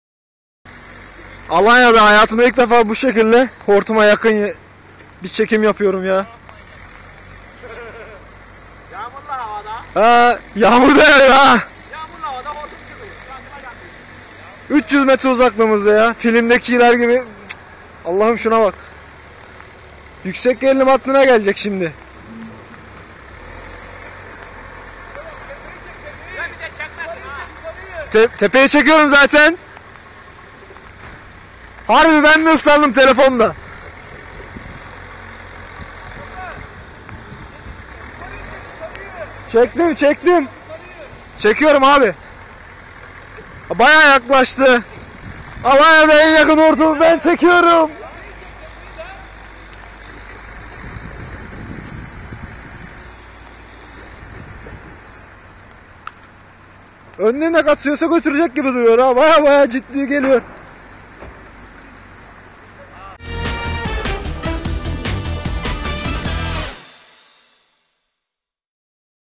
Alanyada Hortum Anı Böyle Görüntülendi
Alanya'yı saran hortum felaketi vatandaşlar tarafından görüntülendi.

Haberini Çek - Bize Gönder - KAZAN!